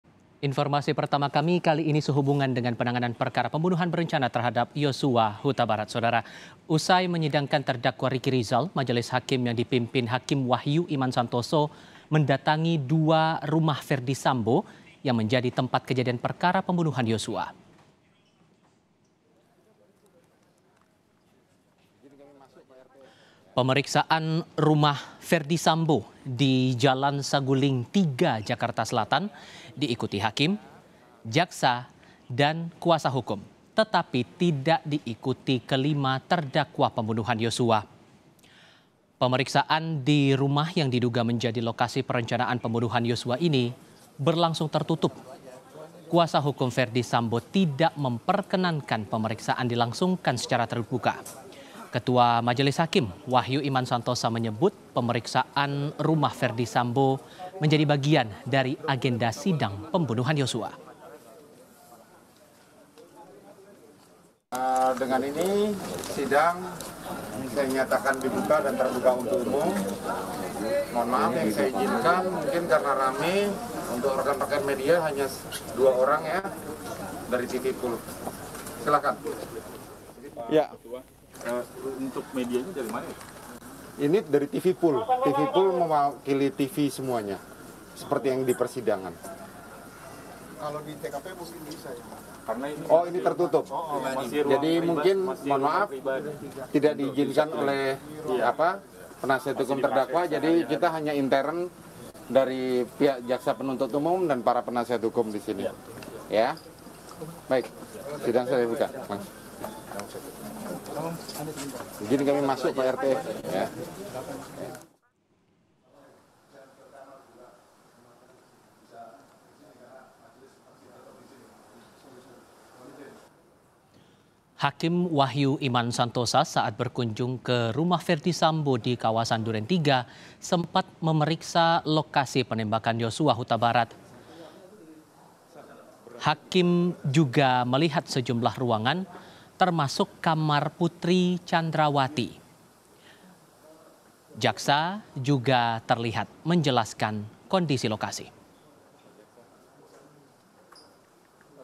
Majelis Hakim Mendatangi 2 Rumah Ferdy Sambo! Apa yang Ingin Diperiksa?
JAKARTA, KOMPAS.TV - Pemeriksaan di rumah yang diduga menjadi lokasi perencanaan pembunuhan Yosua ini, berlangsung tertutup.

Kuasa Hukum Ferdy Sambo tidak memperkenankan pemeriksaan dilangsungkan secara terbuka.

Ketua Majelis Hakim, Wahyu Iman Santosa menyebut, pemeriksaan rumah Ferdy Sambo bagian dari agenda sidang pembunuhan Yosua.

Saat berkunjung ke rumah Ferdy Sambo di kawasan Duren Tiga sempat memeriksa lokasi penembakan Yosua Hutabarat.

Hakim juga melihat sejumlah ruangan termasuk kamar Putri Candrawathi, jaksa juga terlihat menjelaskan kondisi lokasi.

Penasihat Hukum para terdakwa, Ferdy Sambo, Putri Candrawathi, dan Richard Eliezer memberikan sejumlah catatan dari pemeriksaan Hakim terhadap dua rumah Ferdy Sambo.

Pengacara Sambo mengapresiasi Hakim mengabulkan sidang pemeriksaan tempat, dan berharap Hakim memutus perkara sesuai fakta.

Sementara, Pengacara Richard Eliezer Menyoroti, semestinya ada CCTV di Rumah Saguling.

Ia menilai semestinya seluruh terdakwa mengetahui peristiwa pembunuhan Yosua, karena dekatnya jarak di Rumah Duren Tiga.